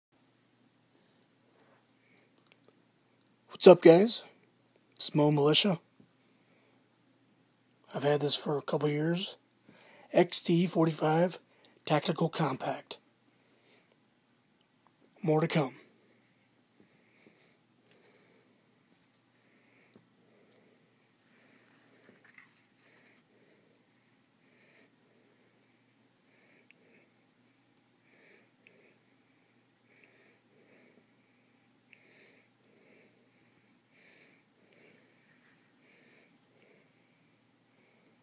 XD .45acp Tactical Compact
I've had this for awhile , it's my Nightstand gun. It's called a "compact" because of the small grip, Tactical because of the 5" slide. It takes 10 round mags flush and 13 round extend mags. It has Trijicon night sights on it and a TLR-4 flashlight / laser on it( although I'm not a fan of lasers I picked it up used for $40) . My local gunsmith did a trigger job , so it's about 3 lb trigger pull.